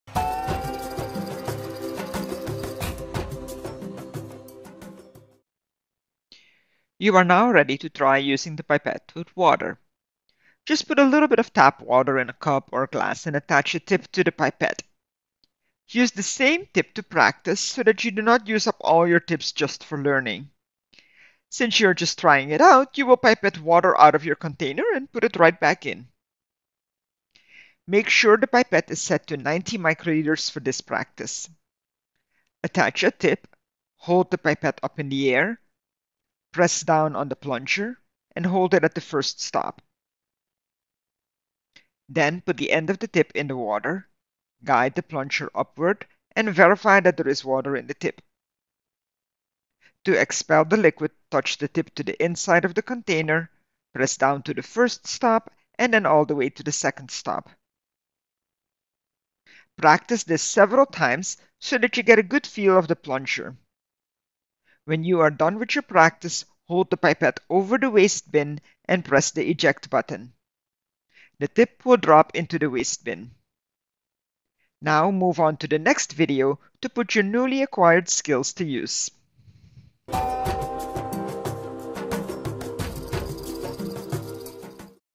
Practice your pipetting skills
This video demonstrates how to practice pipetting.